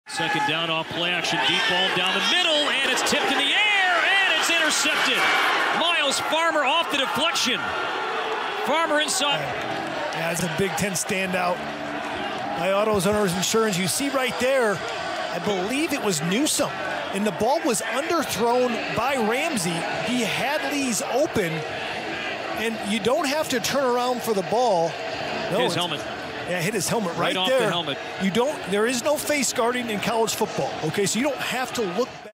DOINK! | Huskers Get the Interception After Pass Ricochets Off Receiver's Helmet | B1G
Nebraska's Myles Farmer comes up with the interception after Northwestern QB Peyton Ramsey's pass bounces off the helmet of his wide receiver.